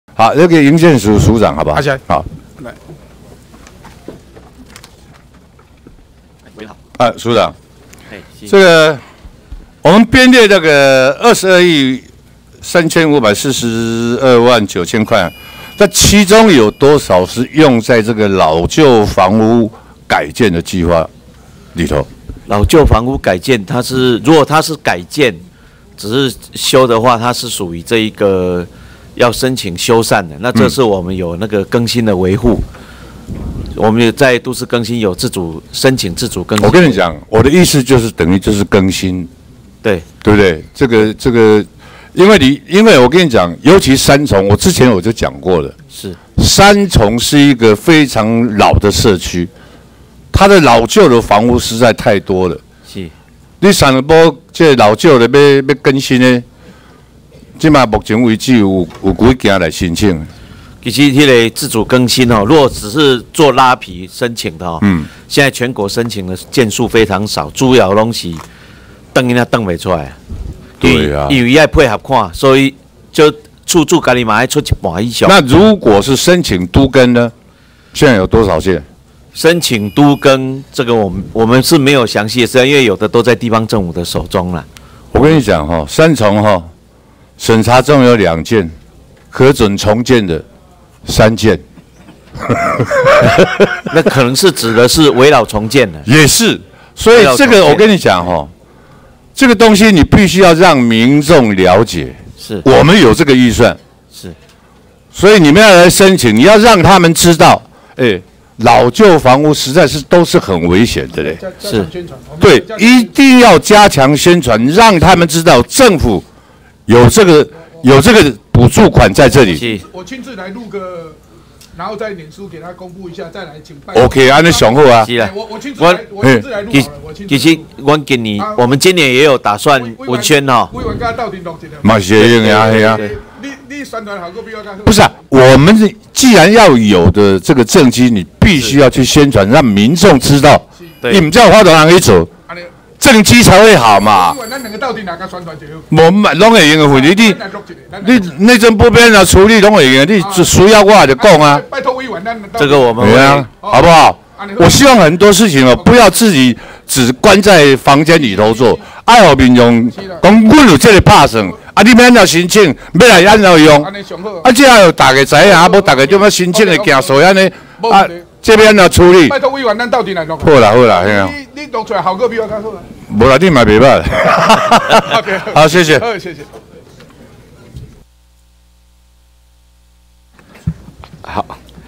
余天 質詢 內政部 目前申請老舊房屋改建有幾件？危老重建補助要讓民眾多了解 2019 04 10 內政 徐國勇 【立法院演哪齣？】
內政部長 徐國勇
內政部營建署署長 吳欣修